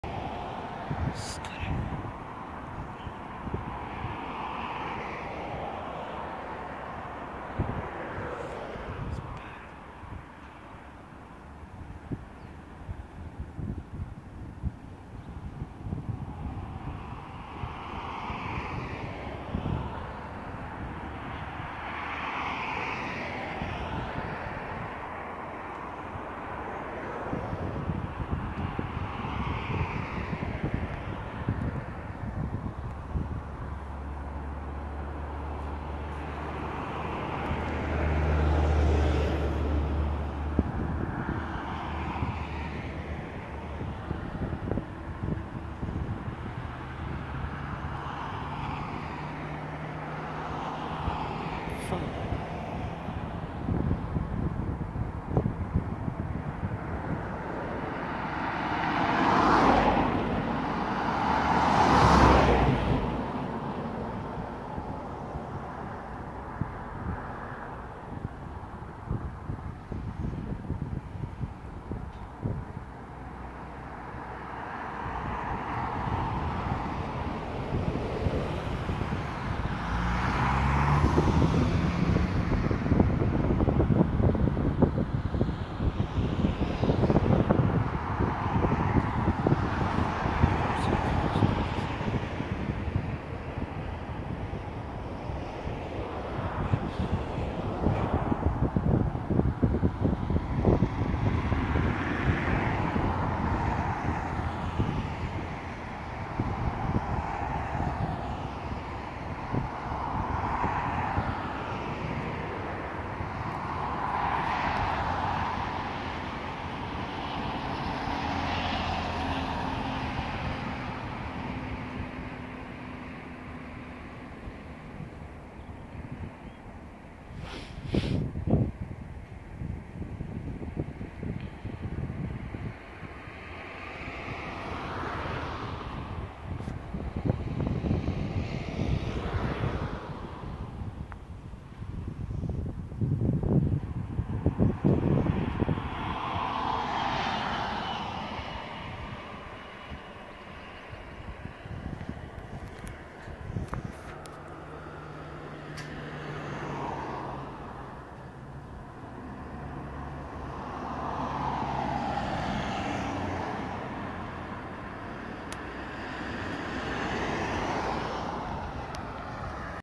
Titan Boeing 767 landing at Cork
Titan airways Boeing 767 collecting the star wars cast from west cork and Kerry where they were filming for the next movie.